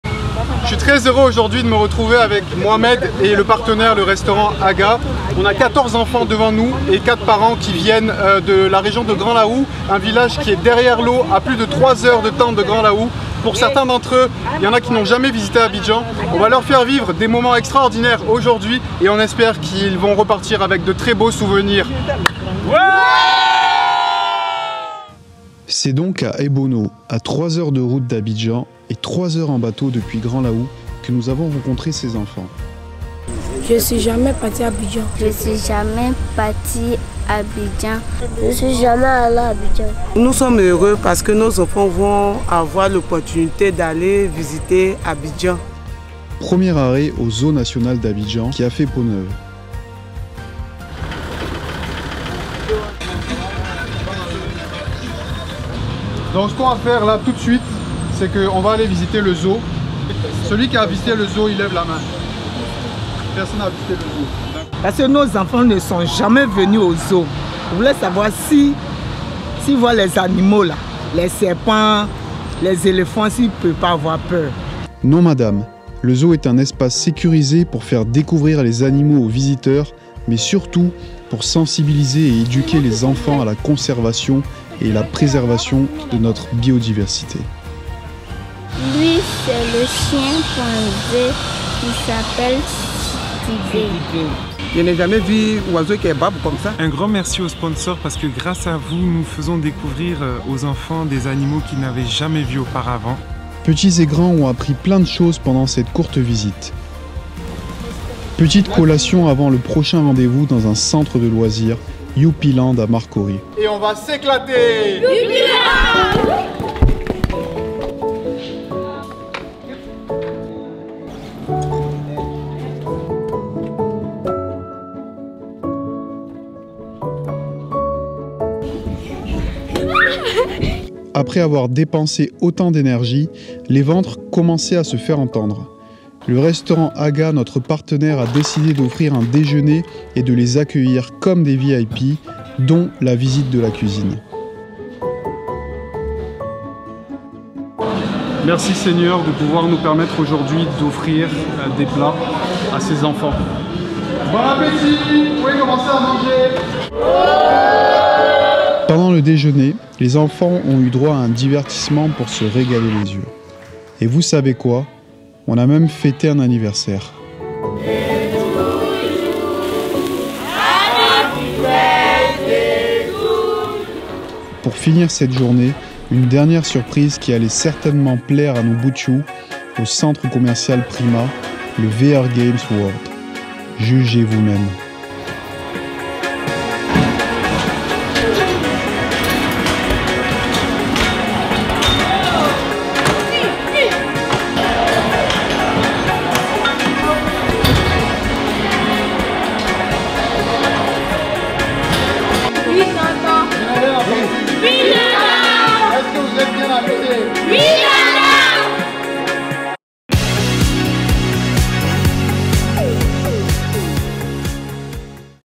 🇨🇮 Abidjan Action sociale | Des enfants du village en ville 🧒🏾
Des enfants du village en ville ! On s'est dit que ça serait génial d'offrir une journée sympathique et amusante à des enfants qui vivent dans un village qui se trouve à 6 heures d'Abidjan. Visite du zoo national d'Abidjan, ...
La suite en vidéo